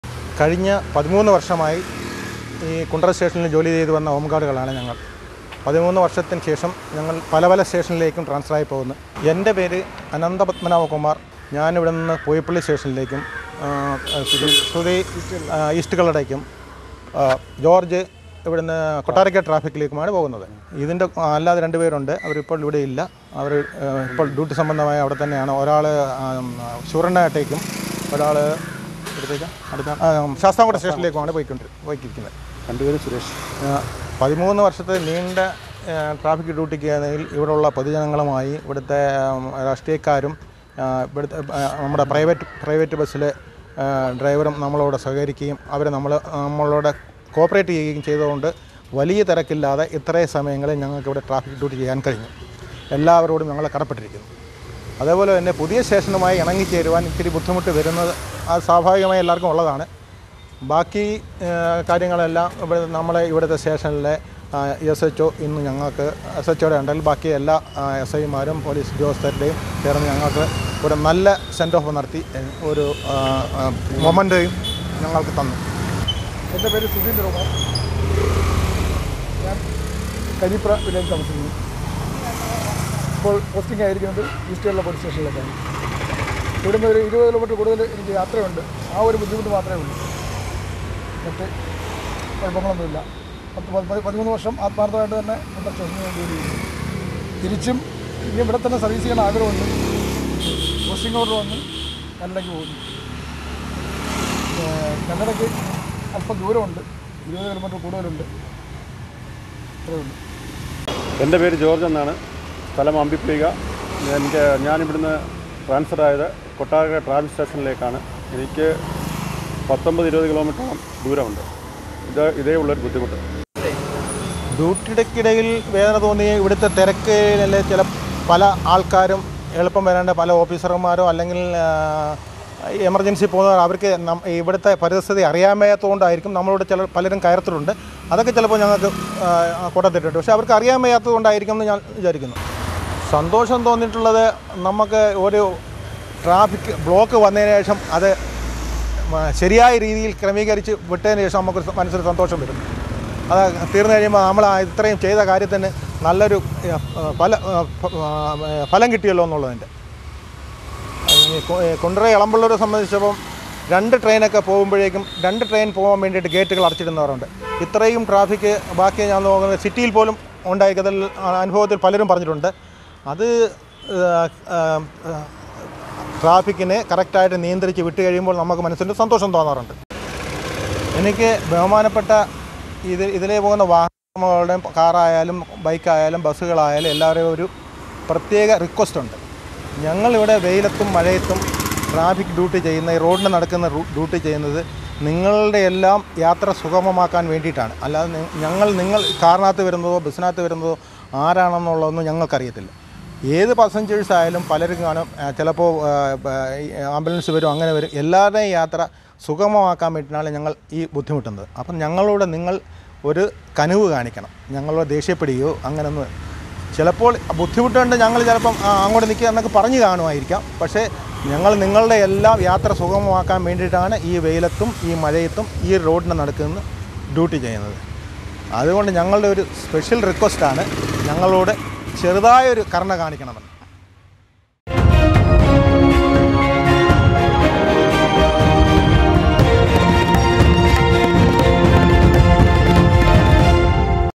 കുണ്ടറയിലെ 13 വർഷത്തെ സേവനത്തിനു ശേഷം സ്ഥലംമാറി പോയ 3 ഹോം ഗാർഡുകൾ അനുഭവങ്ങൾ പങ്കുവെയ്ക്കുന്നു.
കുണ്ടറ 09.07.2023: കഴിഞ്ഞ പതിമൂന്നു വർഷങ്ങളായി കുണ്ടറയിലെ ഇളമ്പള്ളൂർ, മുക്കട, ആശുപത്രിമുക്ക്, പള്ളിമുക്ക് എന്നിവിടങ്ങളിൽ ഗതാഗത തടസ്സങ്ങൾ നിയന്ത്രിക്കുന്നതിനായി വെയിലത്തും മഴയത്തും കർമ്മനിരതരായി  പ്രവർത്തിച്ച കുണ്ടറ പോലീസ് സ്റ്റേഷനിലെ അഞ്ചു ഹോം ഗാർഡുകൾ സ്ഥലം മാറിപ്പോയി.

കുണ്ടറ അമ്പിപ്പൊയ്ക സ്വദേശികളായ ജോർജ് എം, അനന്തപദ്മനാഭകുമാർ, കരീപ്ര സ്വദേശി സുധീന്ദ്രകുമാർ, പെരുമ്പുഴ സ്വദേശി കെ. സുരേഷ്‌കുമാർ, ശാസ്താംകോട്ട സിനിമാപ്പറമ്പ് സ്വദേശിയായ സുരരേഷ്കുമാർ എന്നീ ഹോം ഗാർഡുകൾക്കാണ് സ്ഥലം മാറ്റം കിട്ടിയത്.
അനന്തപദ്മനാഭകുമാർ പൂയപ്പള്ളി പോലീസ് സ്റ്റേഷനിലേക്കും, ജോർജ് എം കൊട്ടാരക്കര ട്രാഫിക്ക് യൂണിറ്റിലേക്കും, സുധീന്ദ്രകുമാർ കല്ലട പോലീസ് സ്റ്റേഷനിലേക്കും, കെ. സുരേഷ്‌കുമാർ ശാസ്താംകോട്ട പോലീസ് സ്റ്റേഷനിലേക്കും, സിനിമാപ്പറമ്പ് സുരരേഷ്കുമാർ ശൂരനാട് പോലീസ് സ്റ്റേഷനിലേക്കും ആണ് സ്ഥലം മാറ്റം കിട്ടിയത്.

Follow us on KUNDARA MEDIA
Facebook | Youtube | Instagram | Website | Threads
വാർത്തകളും വിശേഷങ്ങളും വിരൽത്തുമ്പിൽ